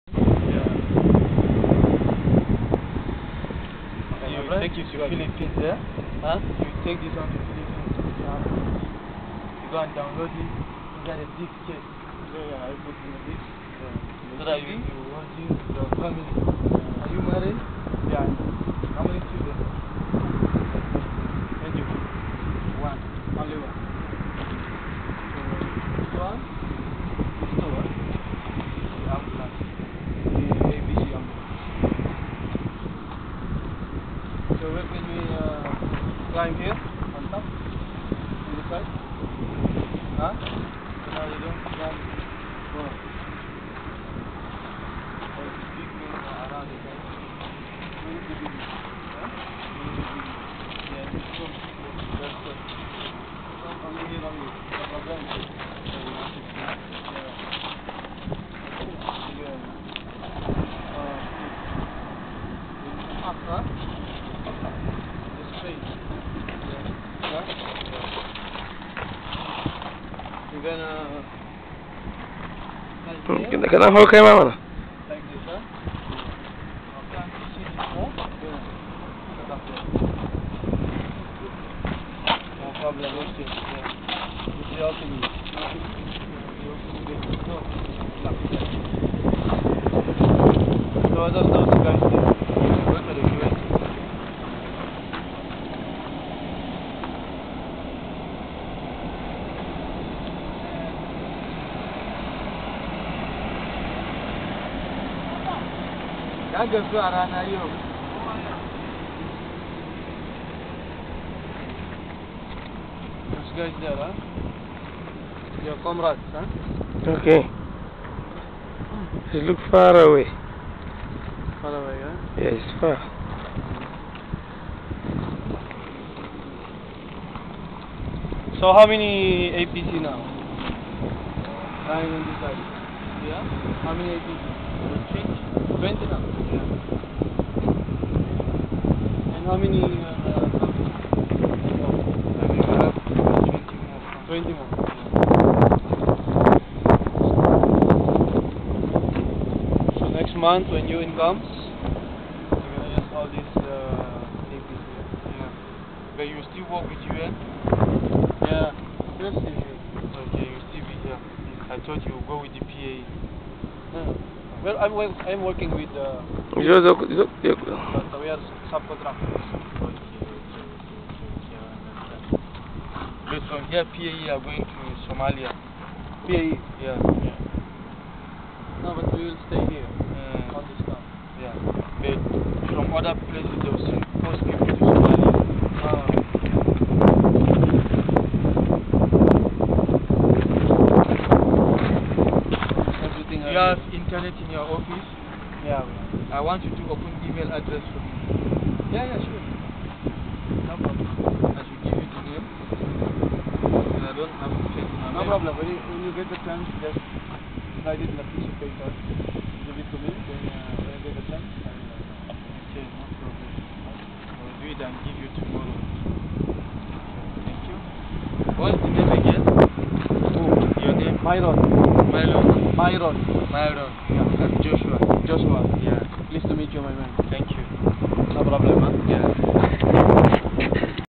Field Medic in Nyala, Darfur, Sudan
Inspecting the APC's with Nigerian Army